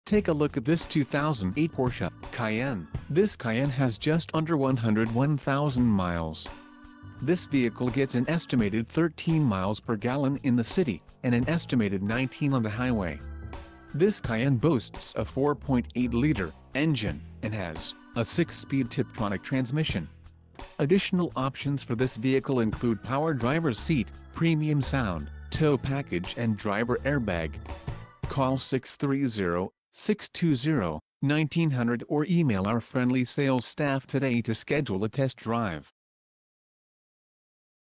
2008 Porsche Cayenne AWD 4dr S SUV - Lombard, IL
International Car Center, Lombard, IL - Porsche Cayenne S V8 SUV AWD Panoramic Roof 20 Sport Design Wheels Park Distance